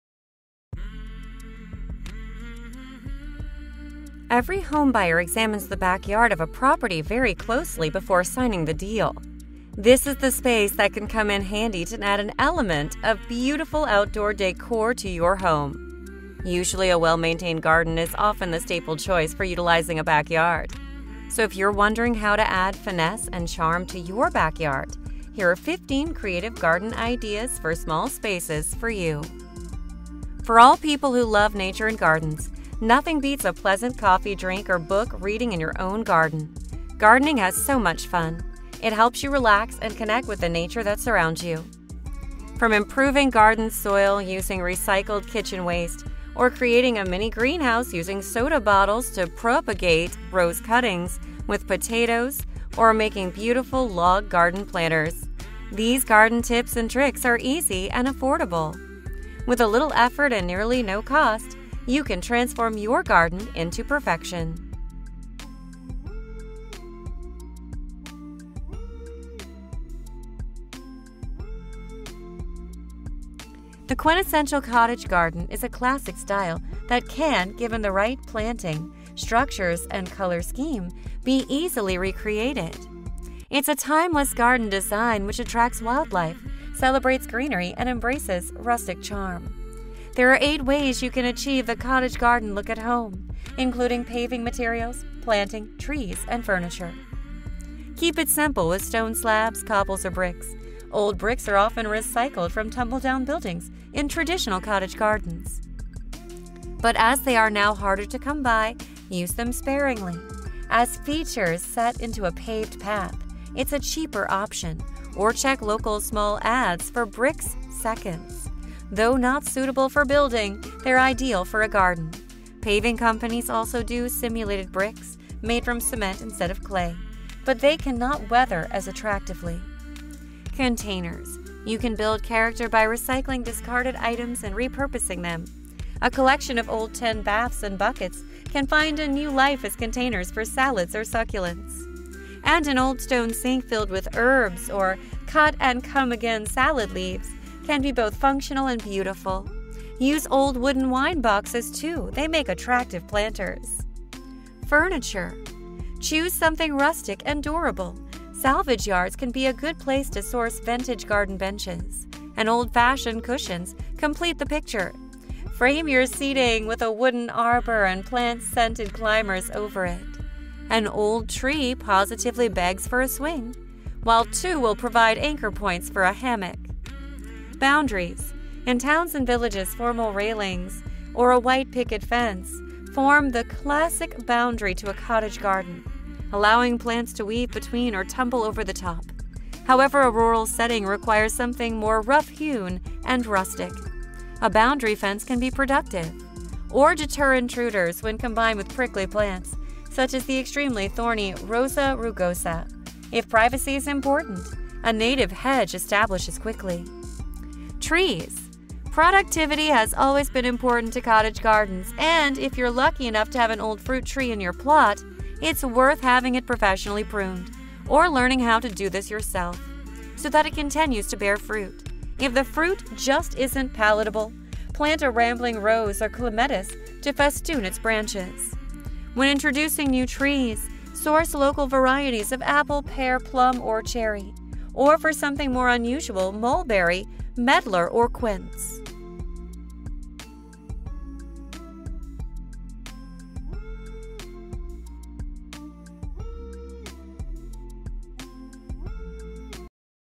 Creative Garden Ideas That Look Way Too Good To Be DIYs
Explore these creative garden ideas to transform your outdoor space into a stunning oasis of beauty and functionality. Whether you have a small backyard or a spacious yard, there's something here for everyone. Discover unique landscaping tips, DIY projects, and innovative gardening techniques that will inspire you to create a personalized and inviting garden. From vertical gardens to fairy-tale pathways, these ideas will help you unleash your creativity and make your garden a place of joy and relaxation. Get ready to turn your garden into a masterpiece!